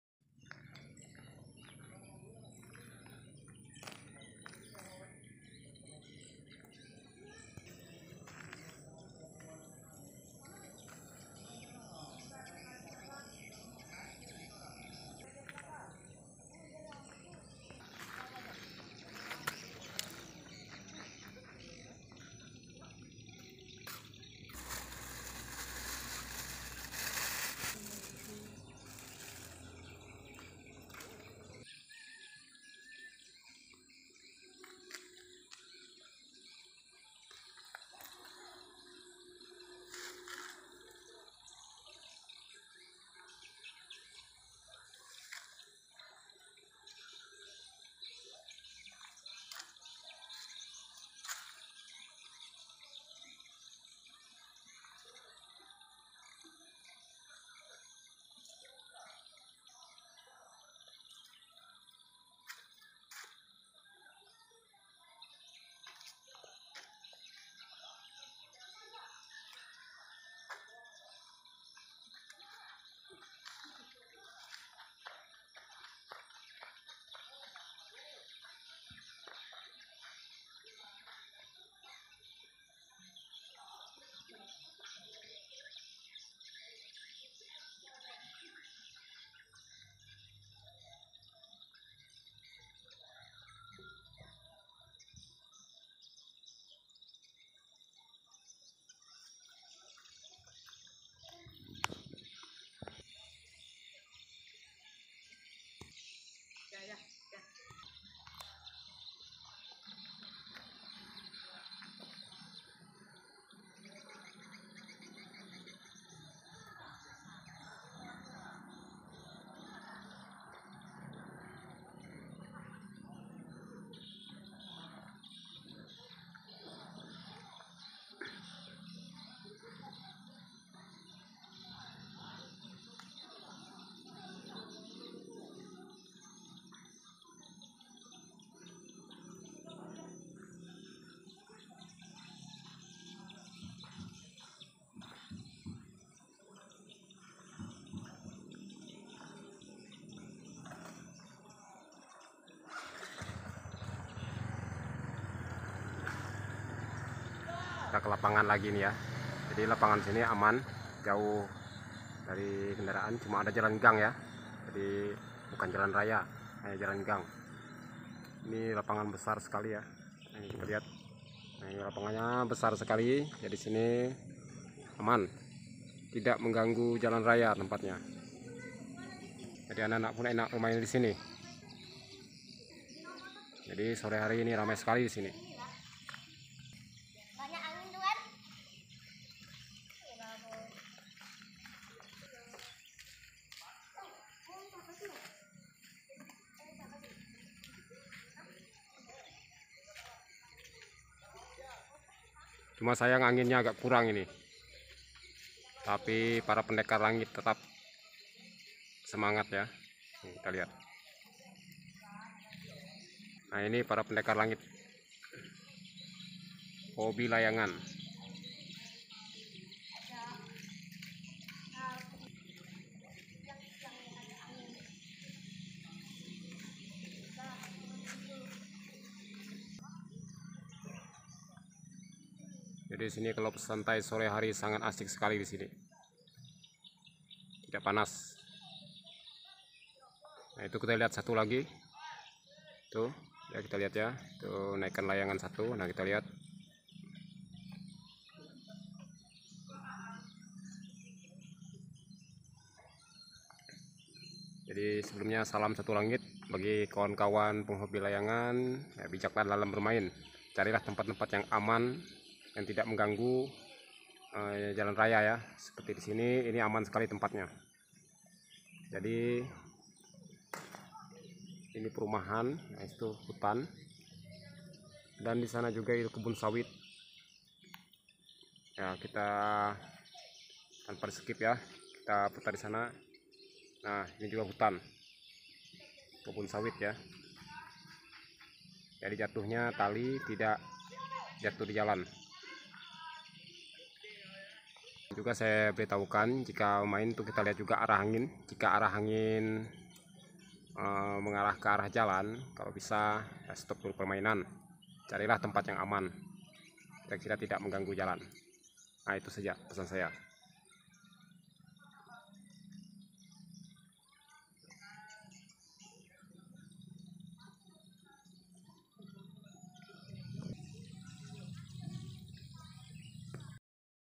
Bertarung diatas langit
Bertarung diatas langit unik layangan wongborneo88 Kali ini saya kembali ke lapangan ketiga untuk menyaksikan adu gelasan layang layang dengan aneka skill untuk bertarung diatas langit dan di sini juga saya akan berikan beberapa tips bermain layangan yang aman dan tidak membahayakan, bagaimanakah keseruan pertarungan kali ini mari kita saksikan video berikut